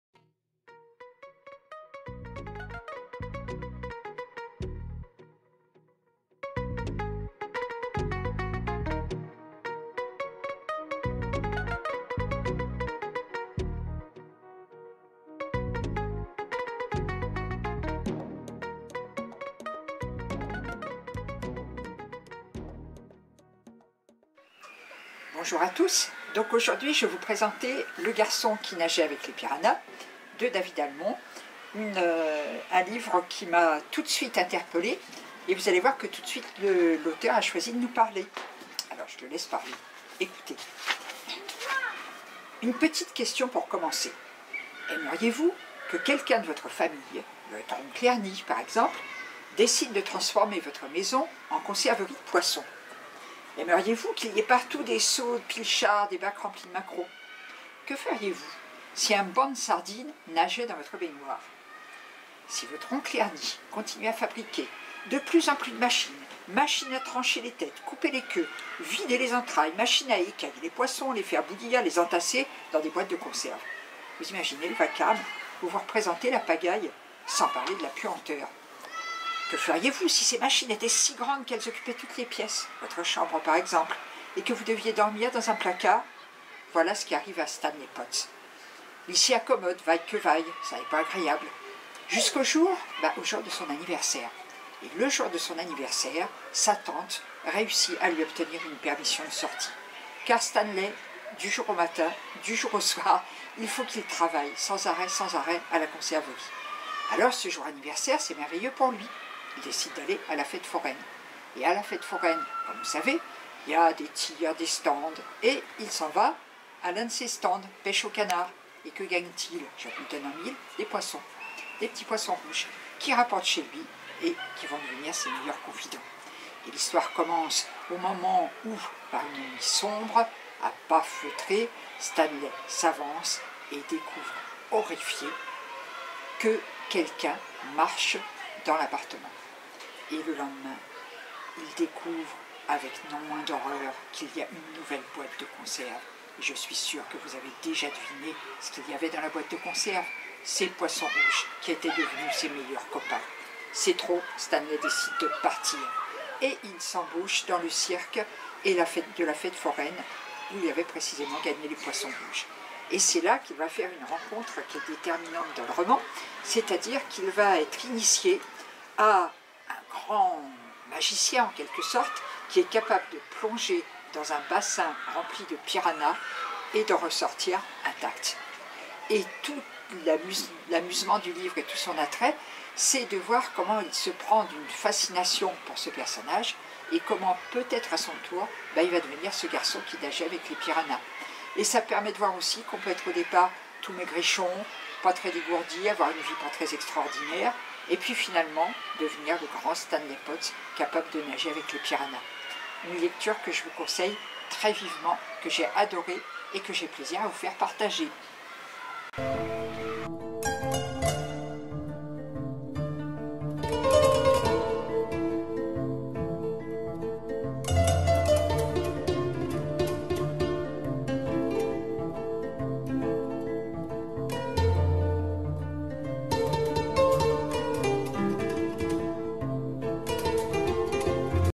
Françoise s'essaye à Critik Book !
Chaque semaine, retrouvez un membre du prix Lire dans le 20e (prix littérature jeunesse des bibliothèques du 20e arr. de Paris) pour vous présenter un livre en course. Aujourd'hui c'est Françoise qui défend de "Le Garçon qui nageait avec les piranhas" de David Almond (Gallimard Jeunesse)